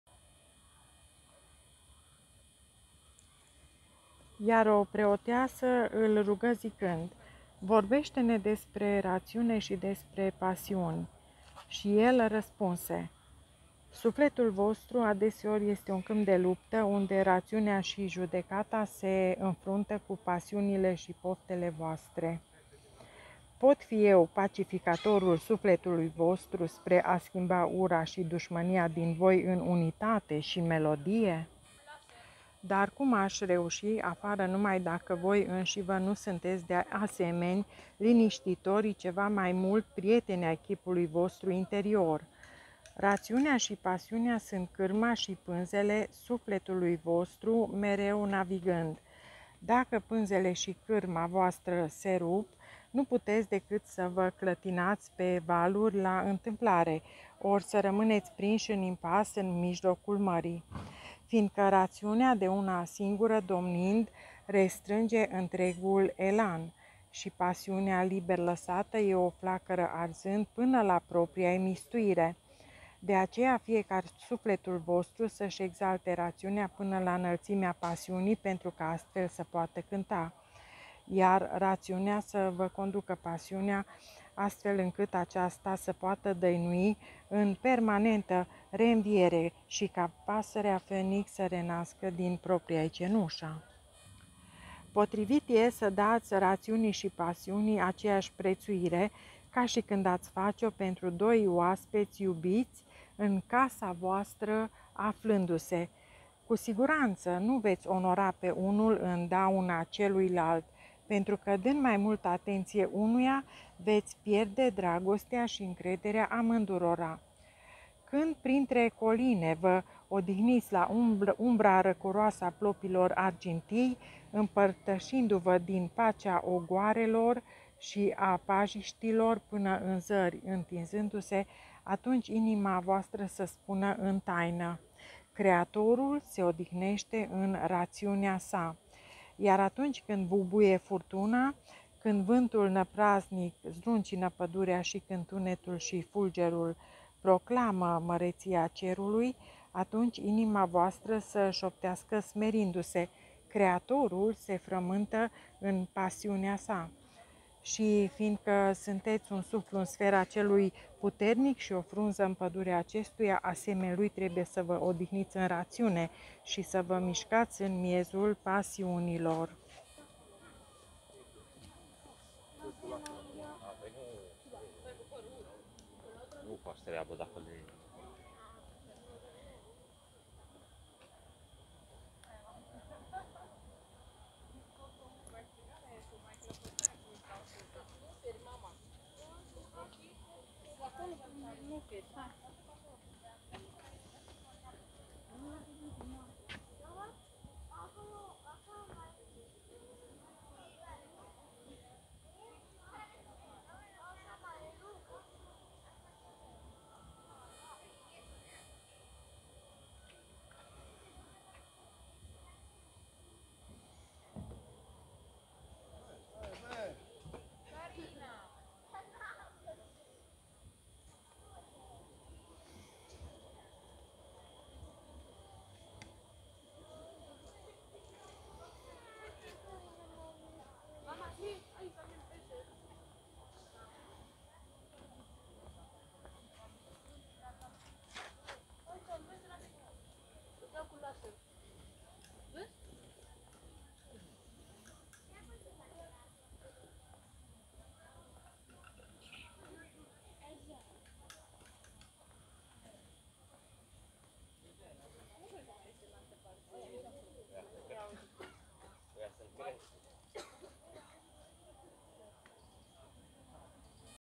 DESPRE RAȚIUNE și DESPRE PASIUNI |15/28| PROFETUL | K. GIBRAN | AudioBook în Română | [v. playlist]
NU CITESC PENTRU BANI!

Cîteva piste mai jos: